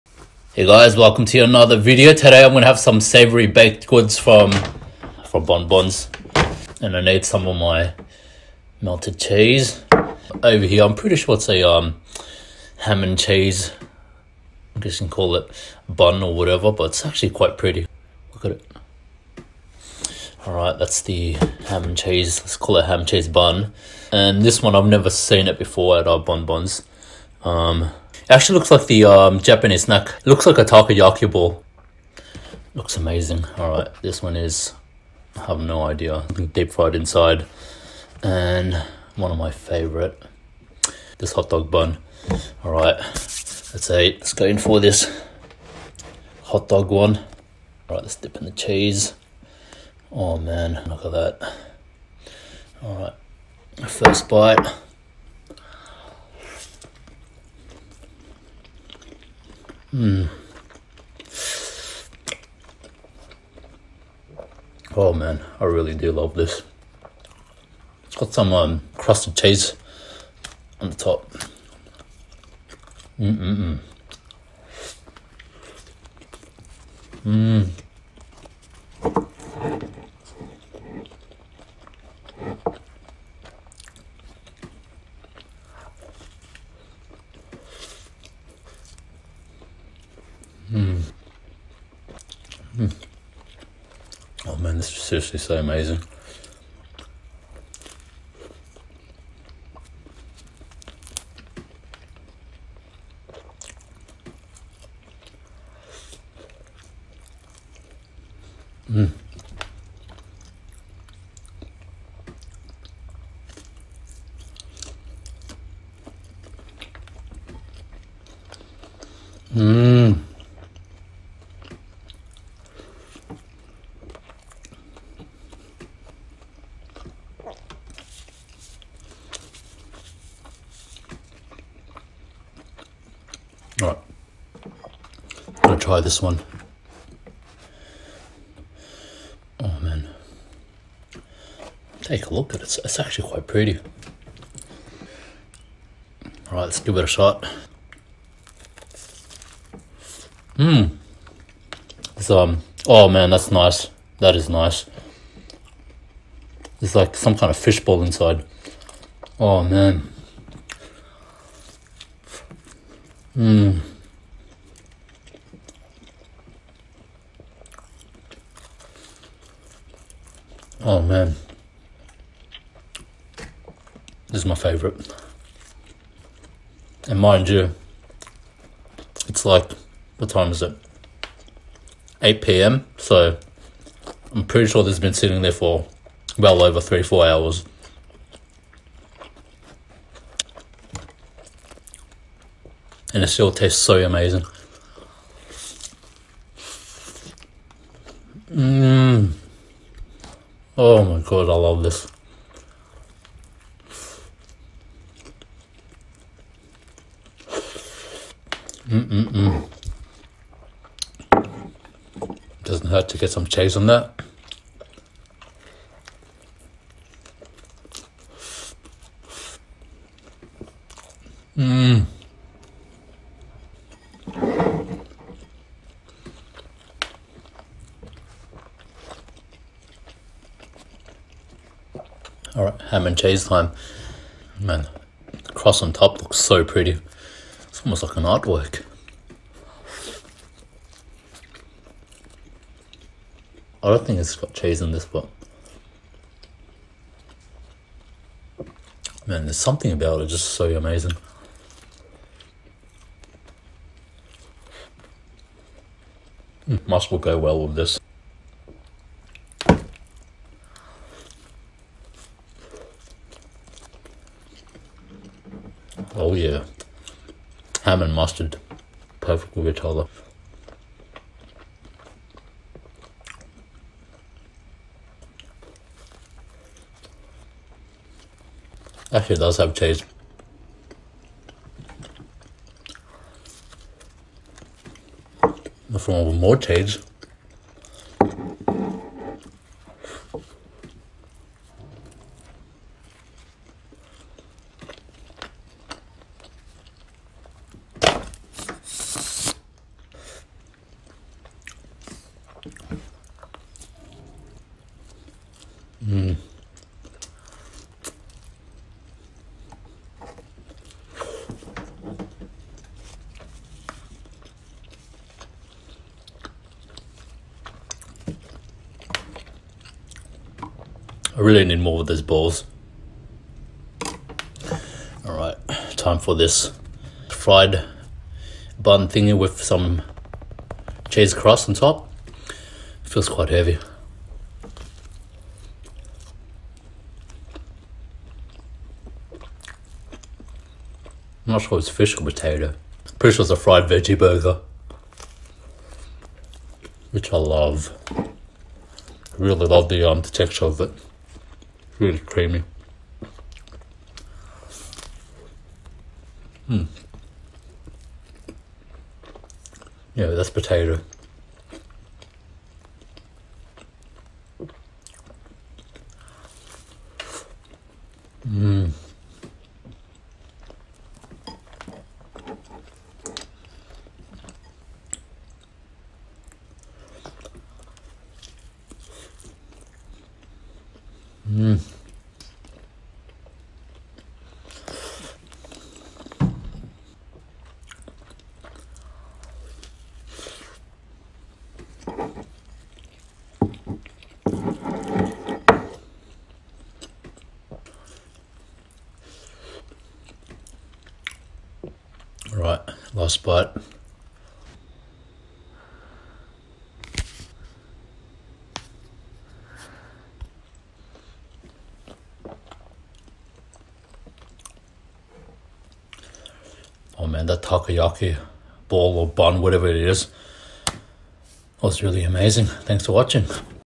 [MUKBANG] SAVORY BAKED GOODS + MELTED CHEESE
Hi everyone, i know I've done a video on this about a 100 times but I'm doing it again XD  I'm eating baked goods from bonbons all savory. i have a hotdog bun, large takoyaki ball, ham and cheese bun, fried potato bun and of course with my melted cheese